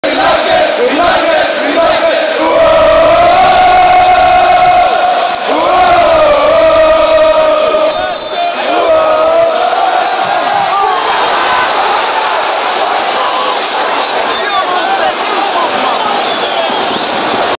N.A.C. - Vitesse (1-2) Panalty(1) Lurling mist
Lurling mist de penalty, 3 minuten later scoort Vitesse aan de andere kant en is die wedstrijd op slot.